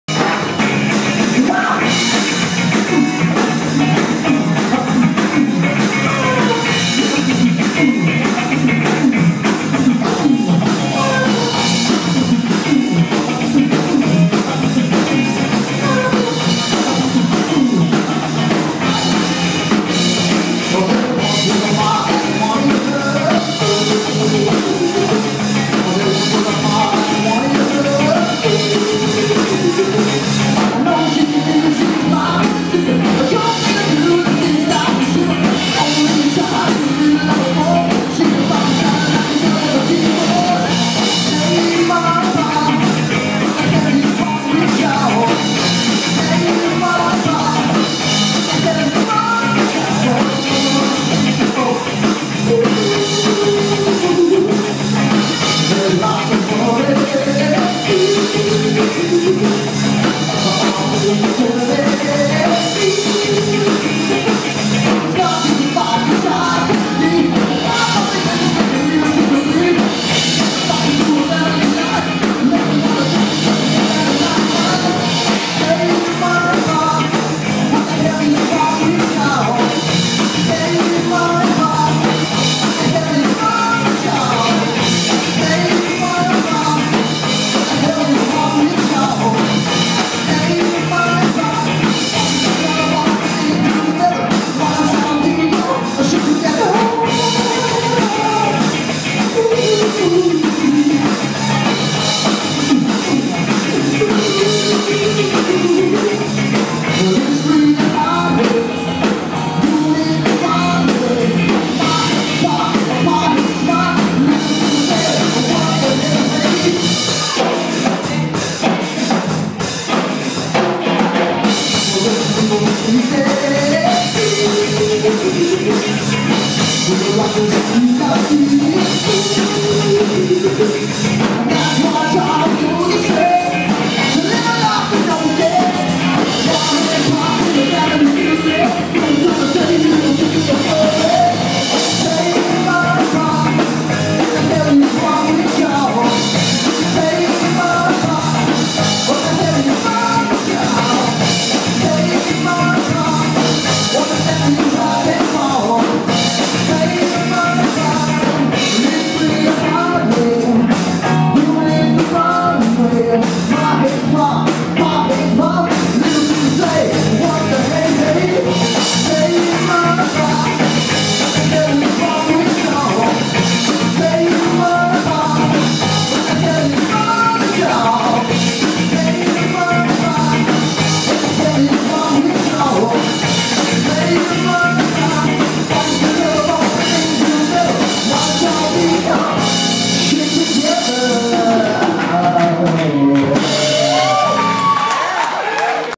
Affordable Lawyers - Live at the Mirkwood Arlington, WA
Live music video of Affordable Lawyers playing live at the Mirkwood Arlington, WA by Hooked on Wax